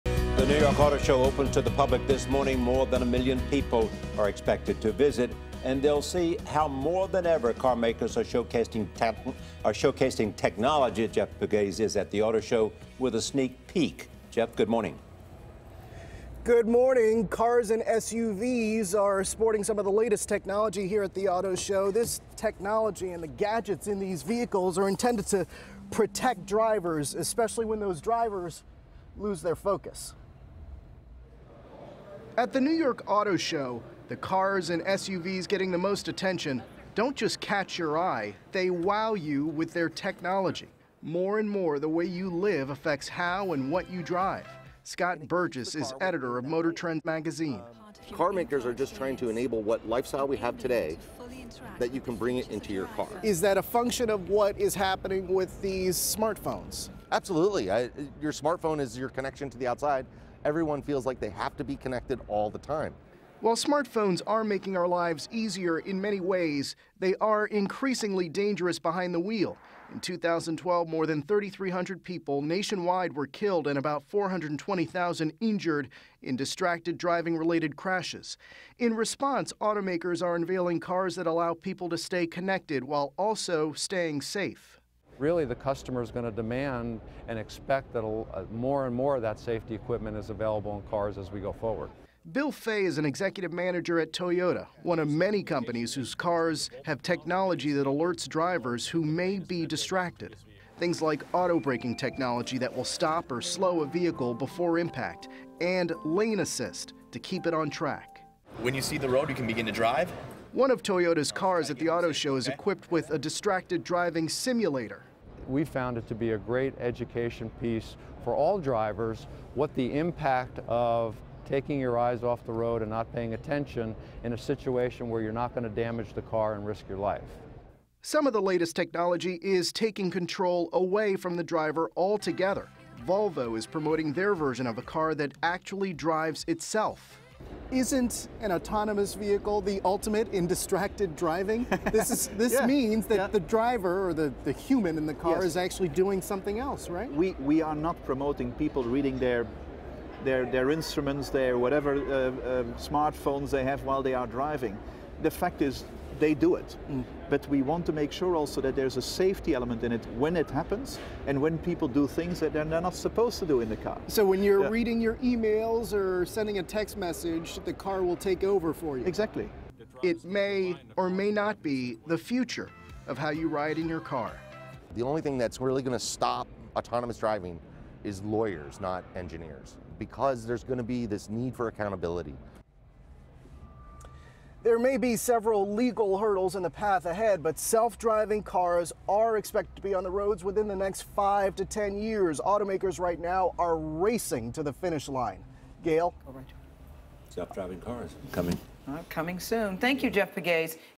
New York Auto Show highlights safety gadgets
Automakers are showcasing the latest technology intended to keep drivers safer behind the wheel while enabling their lifestyles. Jeff Pegues reports.